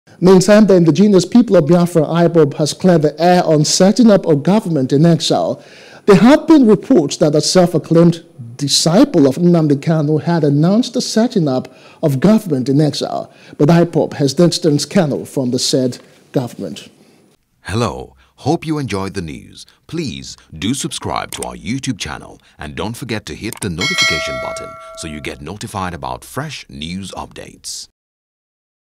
Biafra: IPOB Disowns Setting Up Of Govt. In Exile | NEWS
The Indigenous People of Biafra, IPOB, has  cleared the air on setting up a Government in exile.